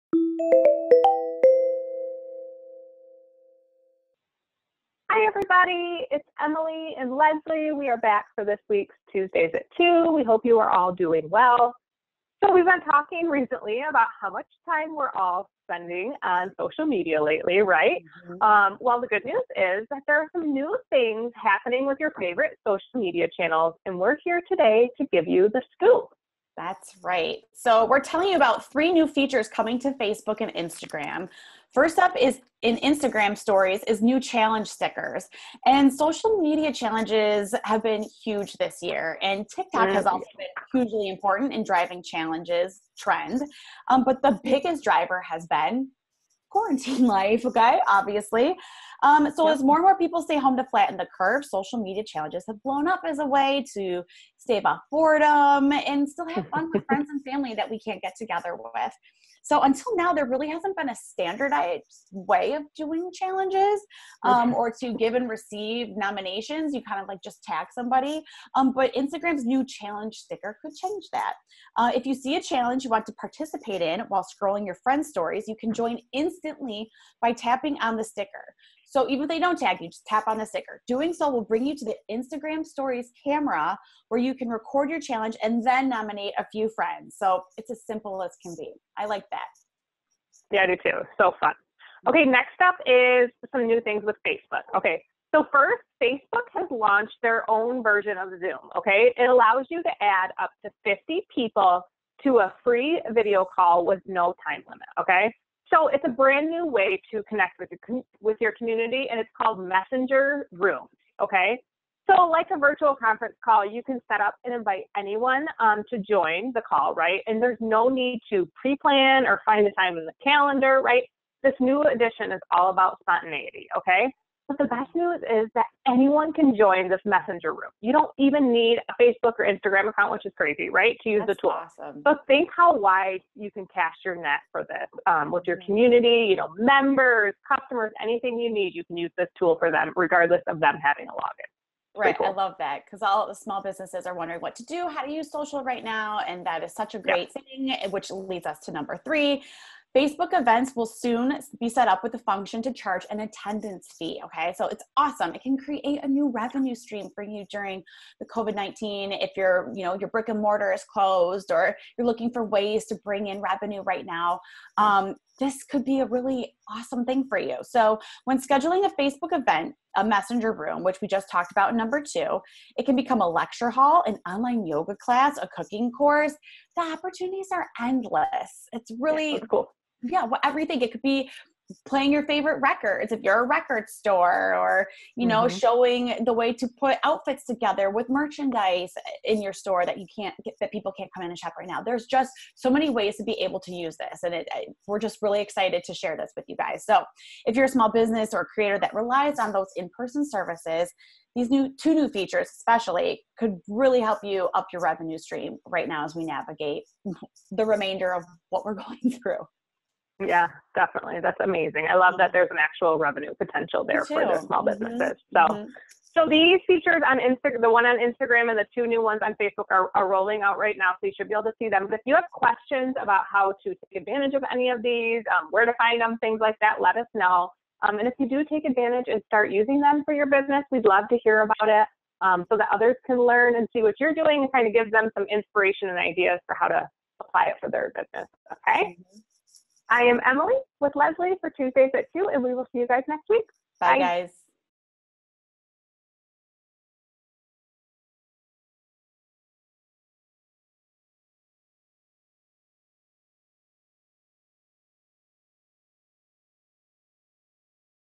3 New Features Coming to Facebook and Instagram
We’ve been talking recently about how much more time we’re all spending on social media due to the pandemic and stay-at-home orders. Well the good news is that there are some new things happening with your favorite social media channels and we are here today to give you the scoop.